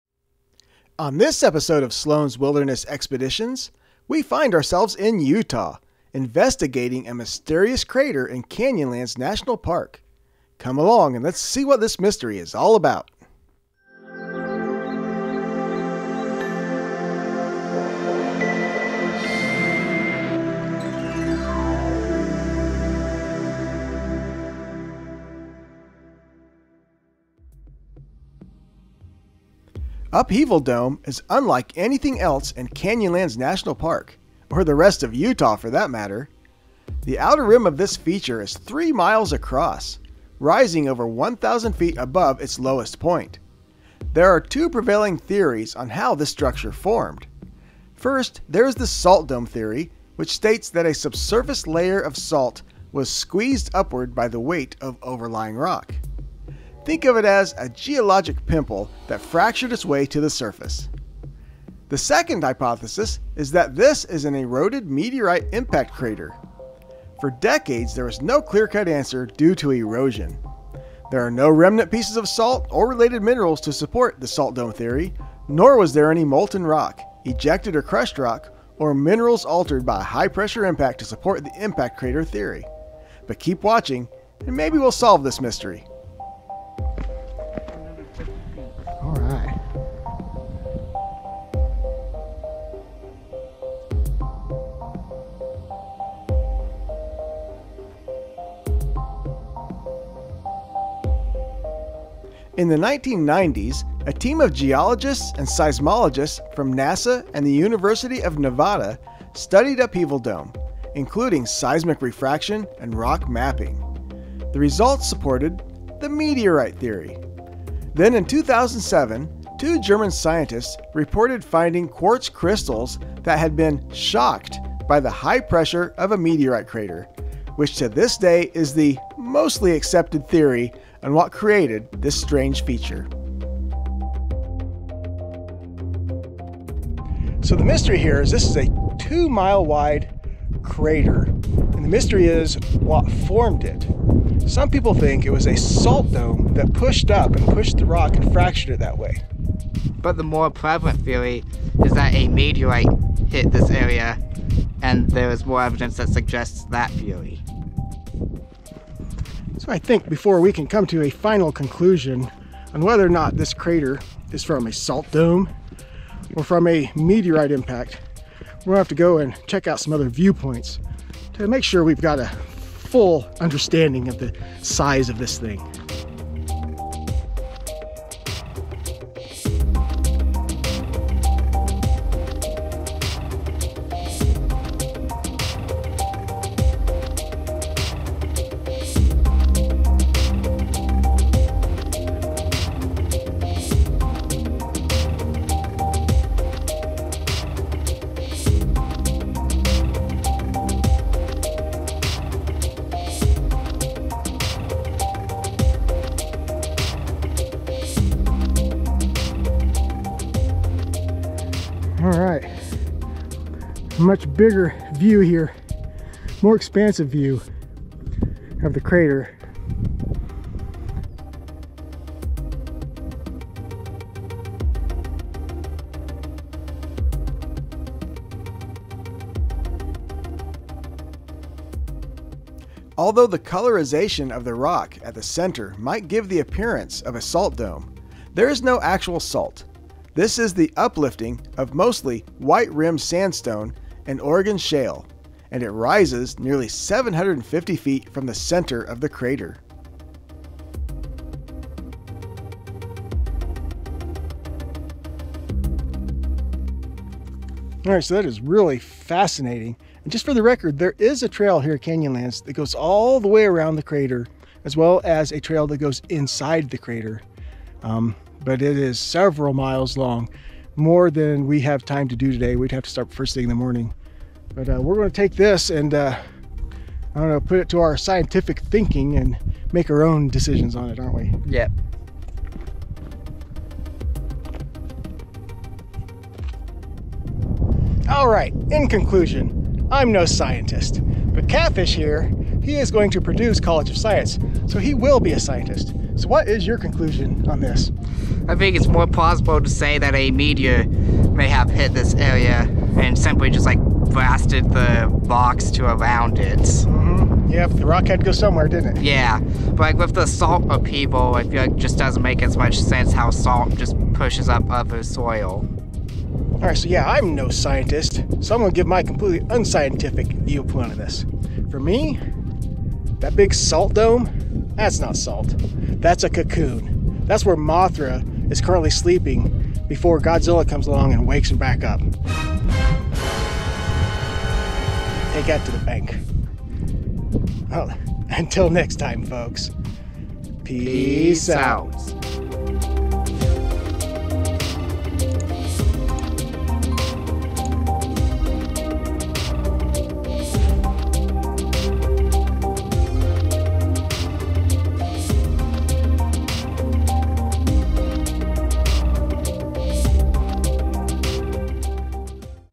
The Mysterious Upheaval Dome | Canyonlands National Park
Inside Canyonlands National Park, near Moab, Utah, is this strange feature that is shrouded in mystery as to how it formed.  It's an enormous crater, with uplifted rock jutting out of it's core.  Is it a salt dome?  Or, is it an ancient meteor crater?   Join us as we explore the mystery of Upheaval Dome.

*This video is non-monetized*

Film Gear:
GoPro Hero 8 Black
Panasonic HC-V180